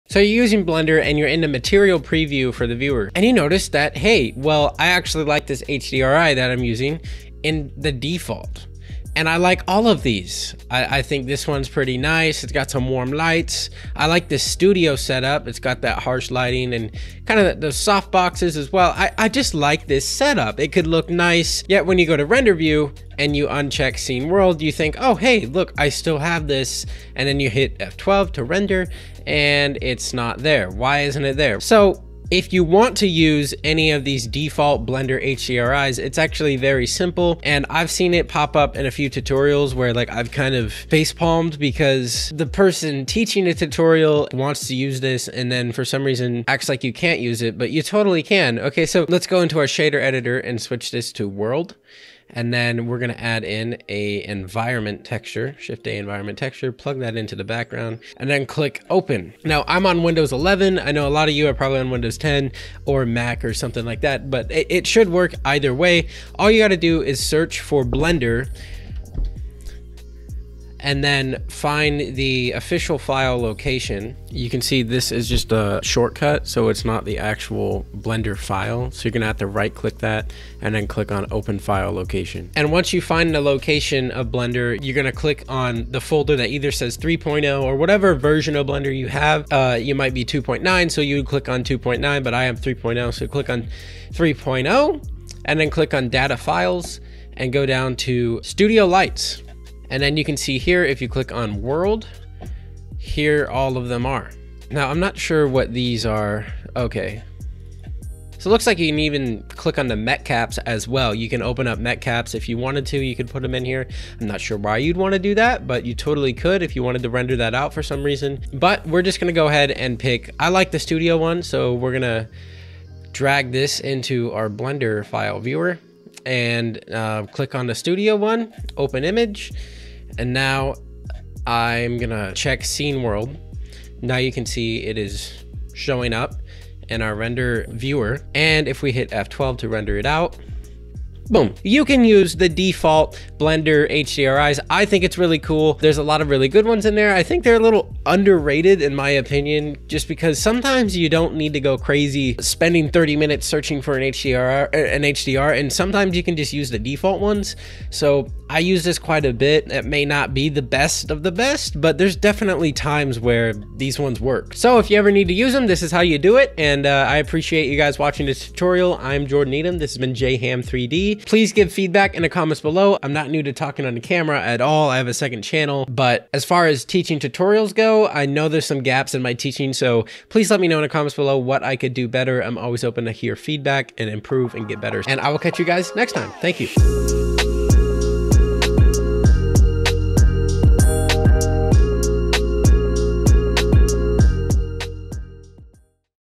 How To Render Blender's Default Worlds Used In Material Viewer (EXRs)
Why won't Blender render the world shown in material preview? Find out how to fix this and use Blender's default worlds!

* Video Correction! *
The term "HDRi" is not interchangeable with "EXR". The files used in this video are EXRs, not HDRs or HDRis. For more on what these different file types mean, this link is helpful

Essentially, they are intended for the same use, but EXRs are typically smaller and should have lossless compression, thus containing more data than HDR file types. There is a lot more to it so I recommend reading the forum :)